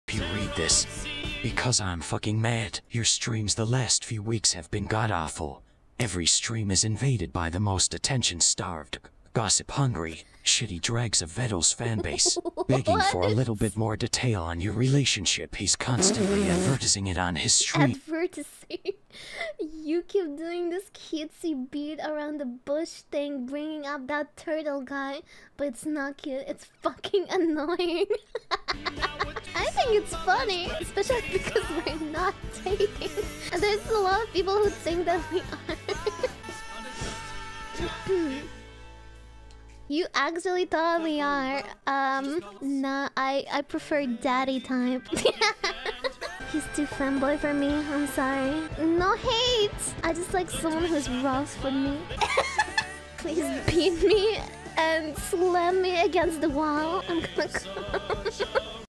*A Vedal "Hater" Donates To Anny*
I hope you read this, because I'm fucking mad. Your streams the last few weeks have been god awful. Every stream is invaded by the most attention starved, gossip hungry, shitty dregs of Vedal's fanbase, begging for a little bit more detail on your relationship. He's constantly advertising it on his stream. You keep doing this cutesy beat around the bush thing, bringing up "that turtle guy". But it's not cute, it's fucking annoying.